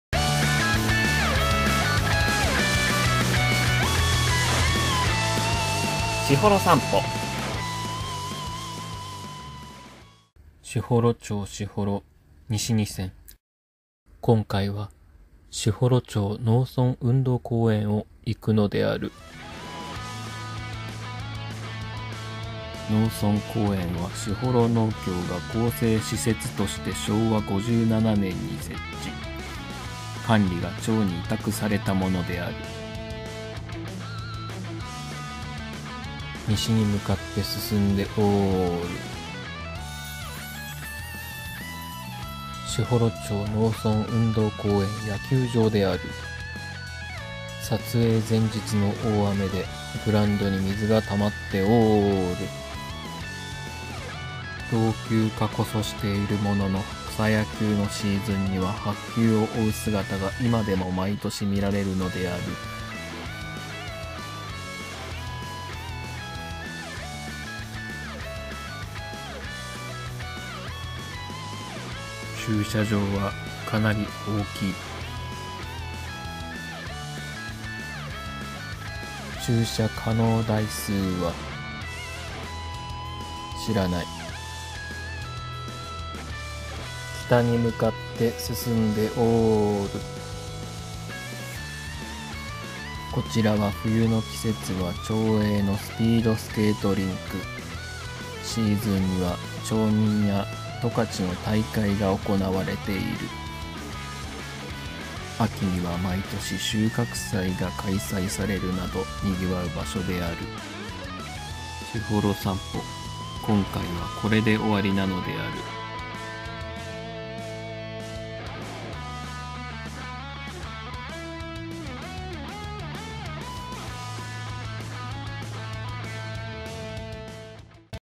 しほろ散歩 士幌町農村運動公園を行く 第4回 (2022.08撮影) 士幌 十勝 北海道
北海道士幌町の道を行く『しほろ散歩』
第4回は、士幌町農村運動公園を行きます。

士幌町農村運動公園は昭和57年に士幌農協が厚生施設として設置。
町に管理が委託されたものです。

野球場やスケートリンクなどがあり、町民の憩いの場のみならず、他にも様々なシーンで活用されています。

※2022年8月に撮影した映像です。